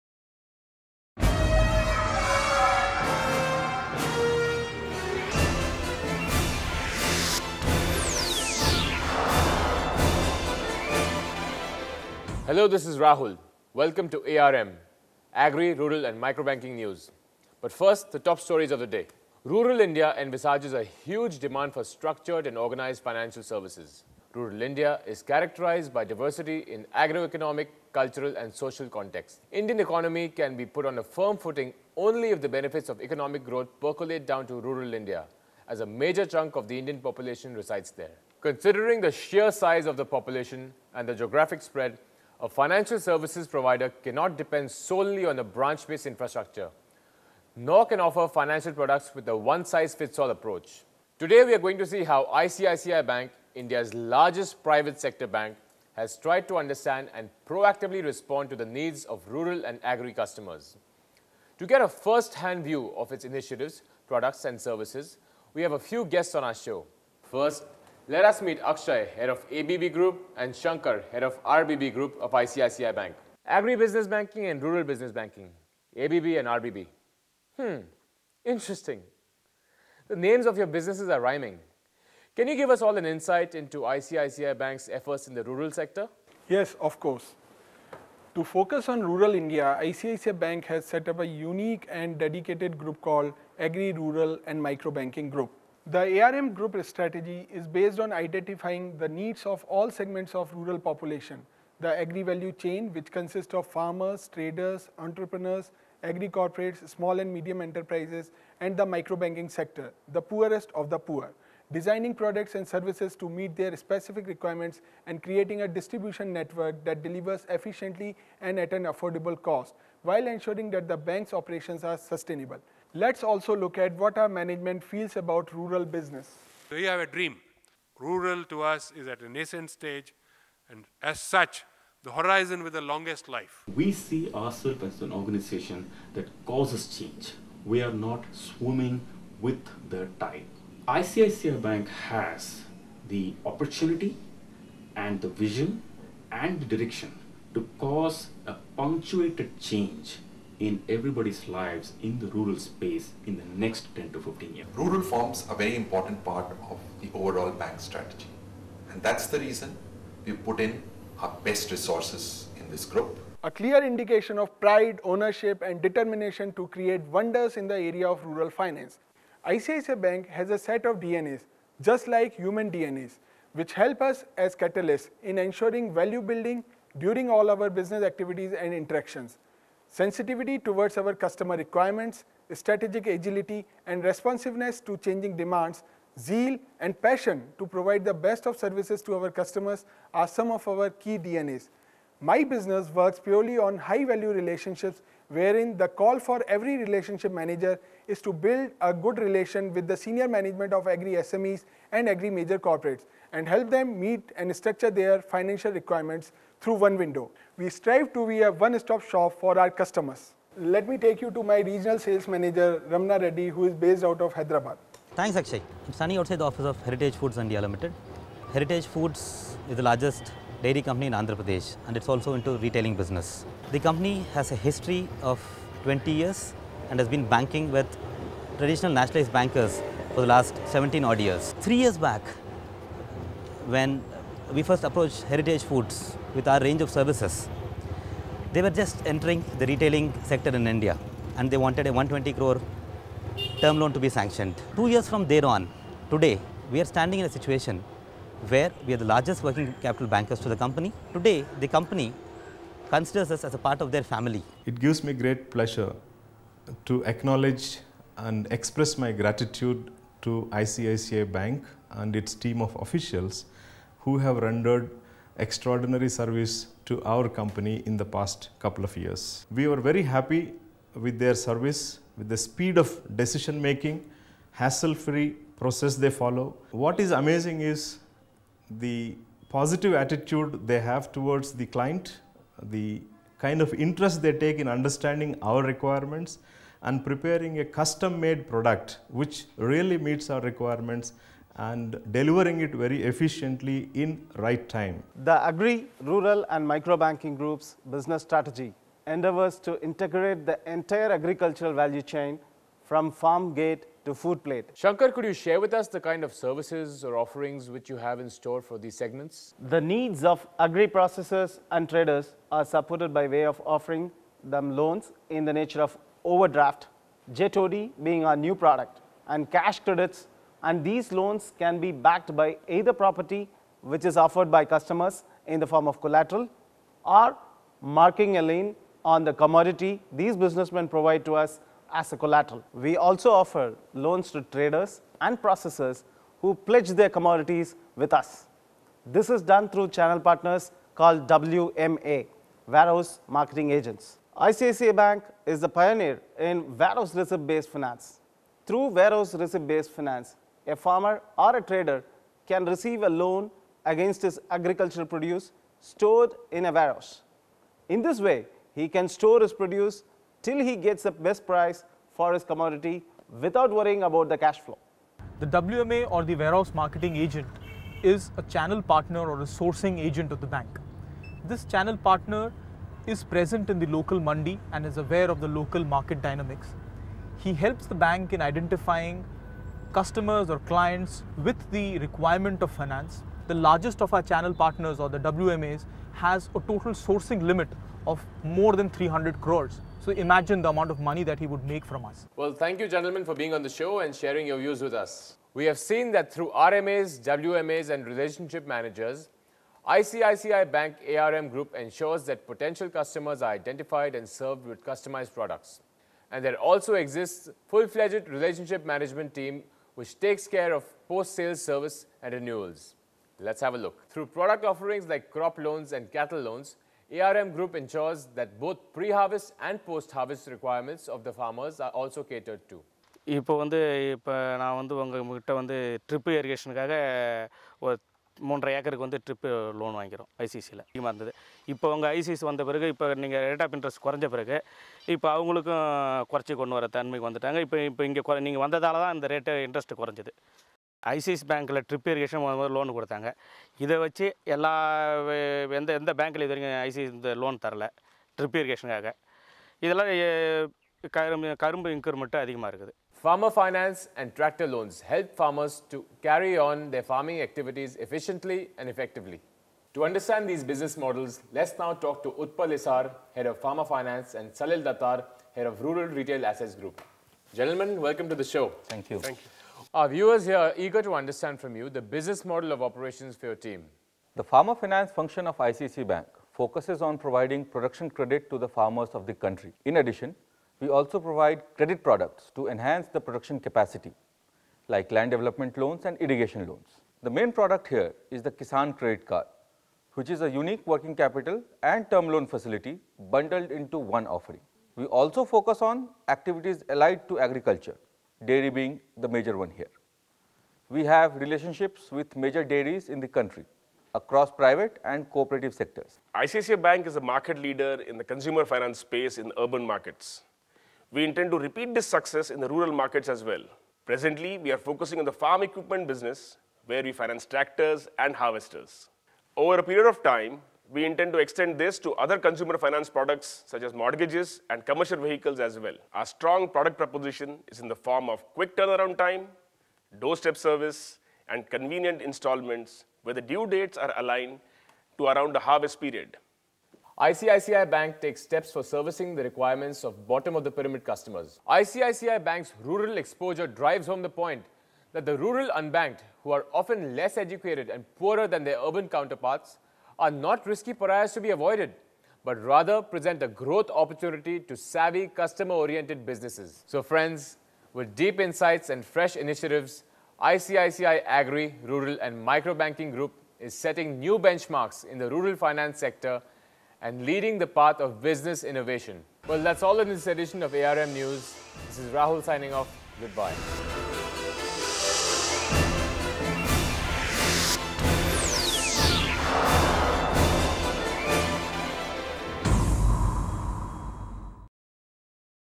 ICICI - Rural Banking Induction AV
A film that gives the viewer a complete feel of the Rural business of ICICI bank. Used for Induction and at familiarization forums.

Credits:
Directed by - Gautam Joglekar
DOP : Raees Ansari
Editor - Tariq Siddiqui